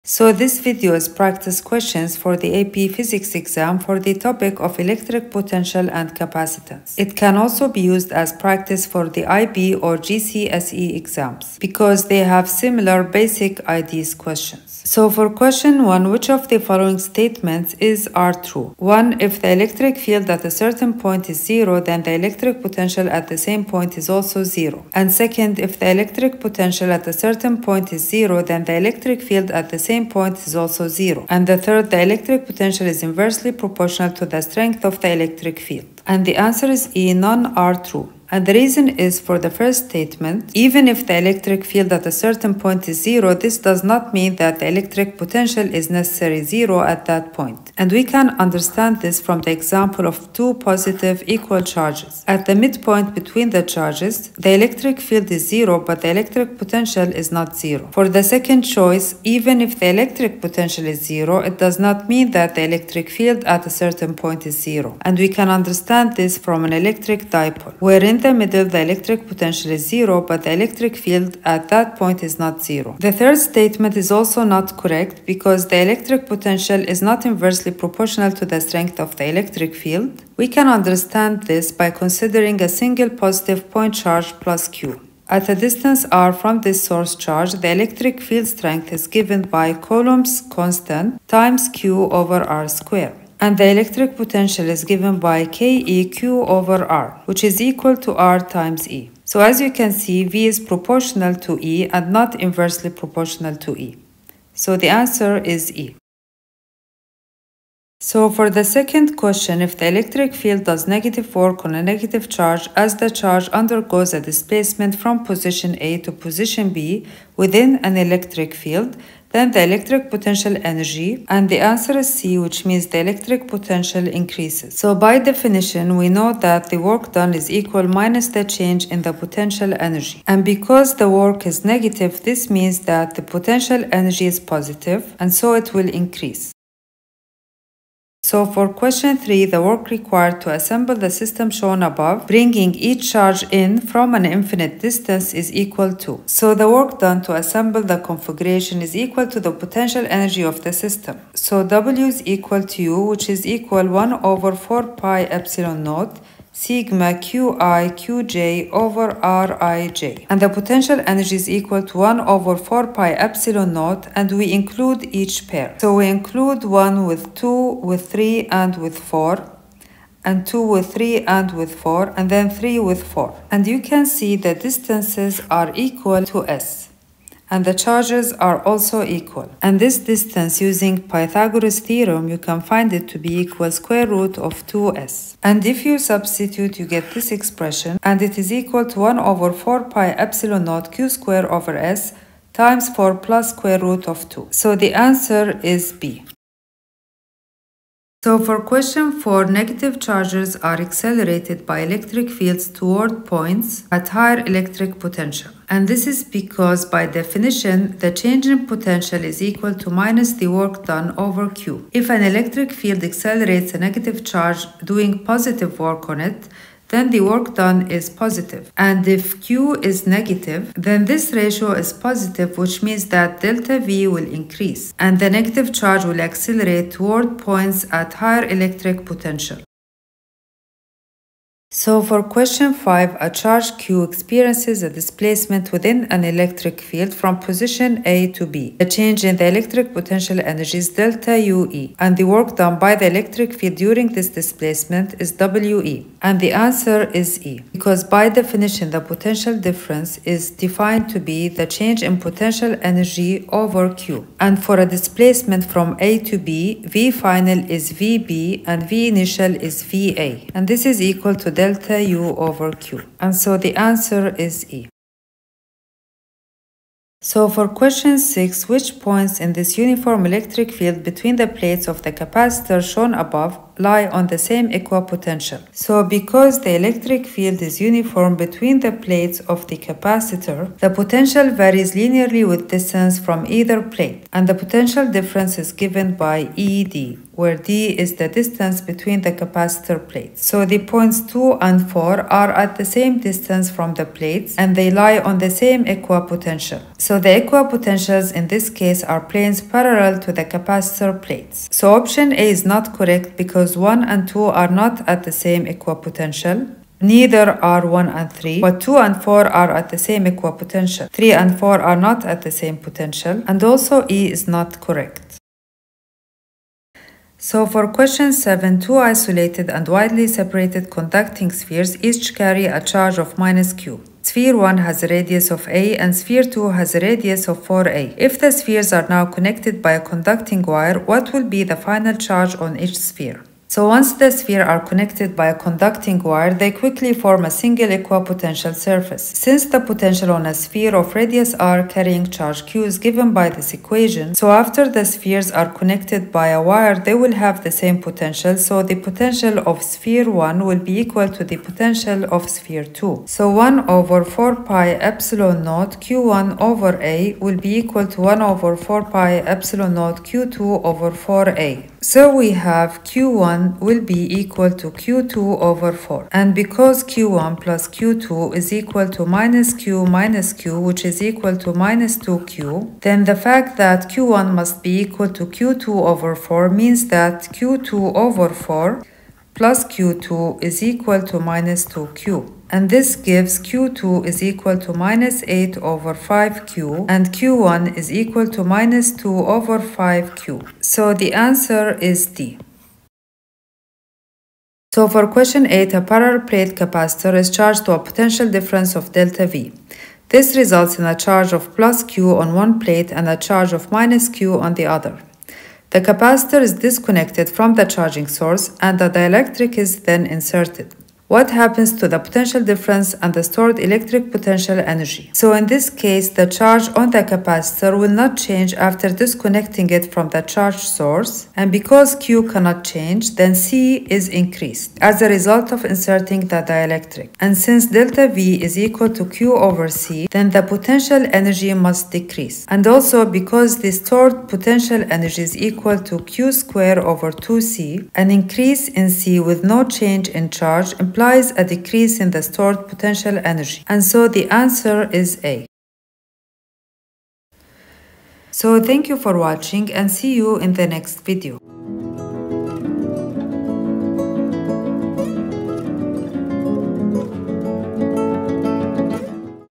AP Physics: Electric Potential and Capacitance Multiple Choice Questions and Answers
This video is from the AP Physics B&C Exams review questions for the topic of Electric Potential and Capacitance. It can also be used as practice for the IB and GCSE exams.

From the book “The Princeton Review: Cracking the AP Physics B&C Exams”